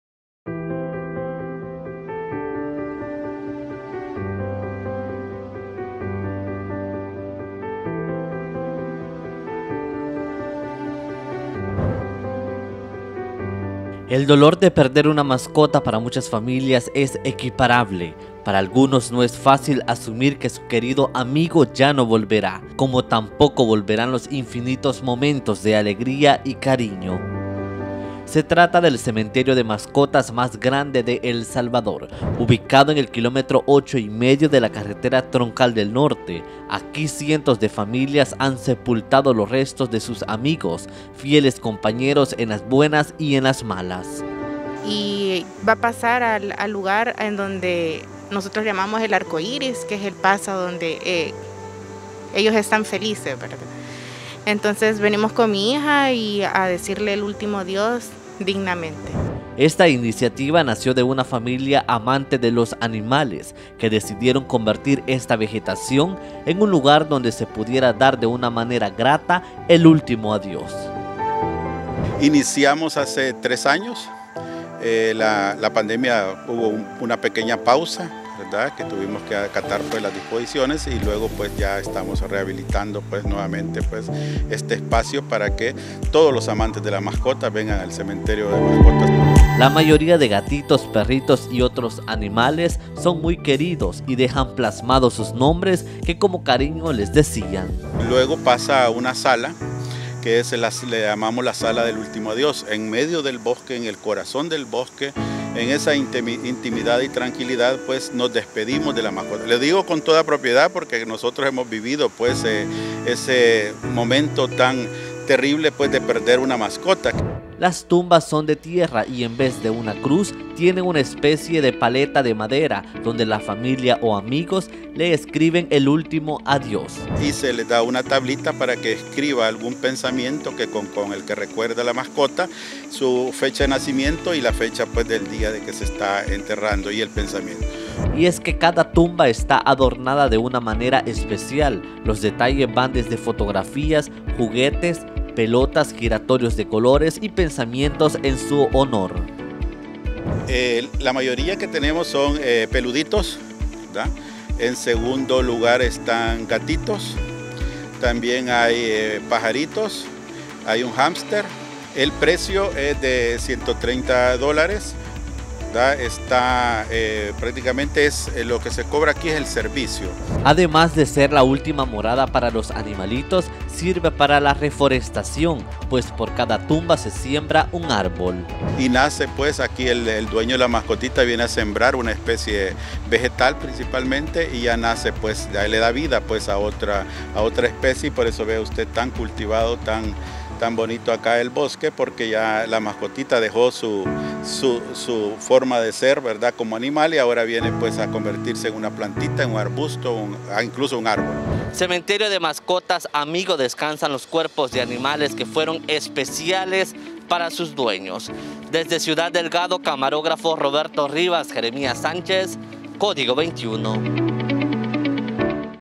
🪦🐾Cementerio de Mascotas🪦
En el país ya existe un lugar donde reposan los restos del mejor amigo del hombre, se trata del cementerio de mascotas, que comenzó a funcionar en 2019 y se ubica en el Kilómetro 8 1/2 de la carretera Troncal del Norte.